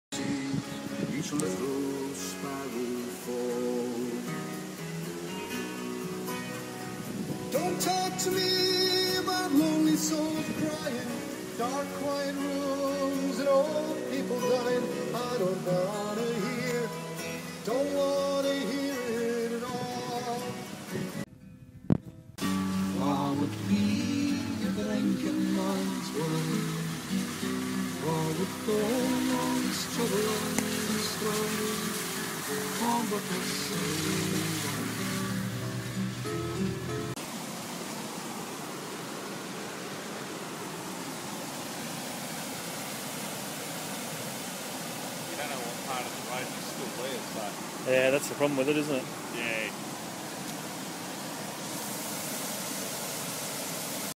2011 Victorian Flash Flooding, Gembrook, Kirth Kiln & Tomahawk Crk
A few vids of the 2011 Flash Flooding in Gembrook forest at Kirth Kiln and around Tomahawk Creek.